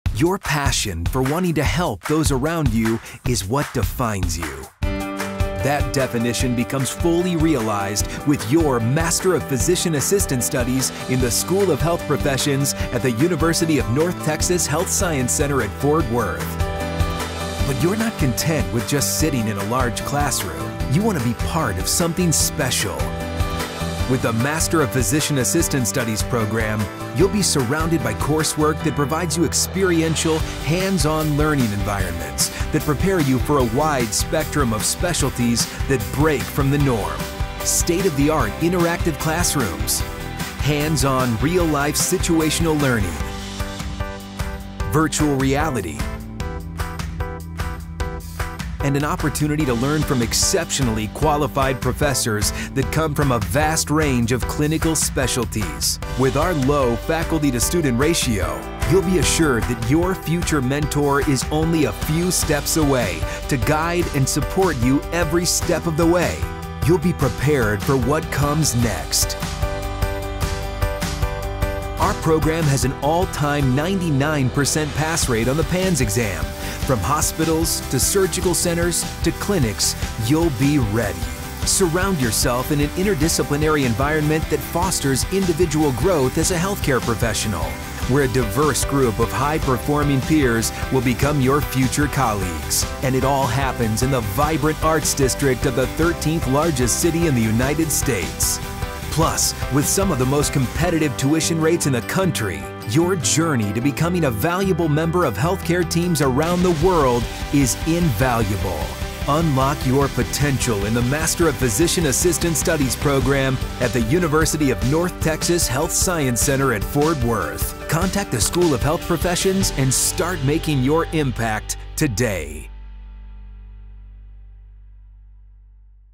The Master of Physician Assistant Program at HSC
The Master of Physician Assistant Program at The University of North Texas Health Science Center at Fort Worth.

0:00 Introduction
0:37 State-of-the-art PA studies
0:55 Our low faculty-to-student ratio
1:11 Our all-time 99% pass rate on the PANCE exam
1:39 Our low and competitive tuition

Discover the benefits of the Master of Physician Assistant Studies program at The University of North Texas Health Science Center at Fort Worth. This video walks you through the program designed for hands-on learners eager to excel in various medical specialties. With a low faculty-to-student ratio, you will get personalized support and mentoring. Plus, our 99% PANCE exam pass rate ensures you will be well-prepared for a fulfilling career in diverse health care settings such as hospitals and clinics. Set in Fort Worth’s vibrant Cultural District and offering competitive tuition rates, this program provides the perfect environment to grow as a health care professional. Watch now to find out how you can take the next step in your medical career.

Video Script:

Your passion for wanting to help those around you is what defines you. That definition becomes fully realized with your Master of Physician Assistant Studies in the School of Health Professions at The University of North Texas Health Science Center at Fort Worth.

But you're not content with just sitting in a large classroom. You want to be part of something special. With the Master of Physician Assistant Studies program, you'll be surrounded by coursework that provides you experiential hands-on learning environments that prepare you for a wide spectrum of specialties that break from the norm.

State-of-the-art interactive classrooms, hands-on real-life situational learning, virtual reality, and an opportunity to learn from exceptionally qualified professors that come from a vast range of clinical specialties.

With our low faculty-to-student ratio, you'll be assured that your future mentor is only a few steps away, to guide and support you every step of the way. You'll be prepared for what comes next.

Our program has an all-time 99 percent pass rate on the PANCE exam. From hospitals to surgical centers, to clinics, you'll be ready.

Surround yourself in an interdisciplinary environment that fosters individual growth as a health care professional. Where a diverse group of high-performing peers will become your future colleagues.

And it all happens in the vibrant Arts District of the 13th largest city in the United States.

Plus, with some of the most competitive tuition rates in the country, your journey to becoming a valuable member of health care teams around the world is invaluable.

Unlock your potential in the Master of Physician Assistant Studies program at The University of North Texas Health Science Center at Fort Worth.

Contact the School of Health Professions and start making your impact today.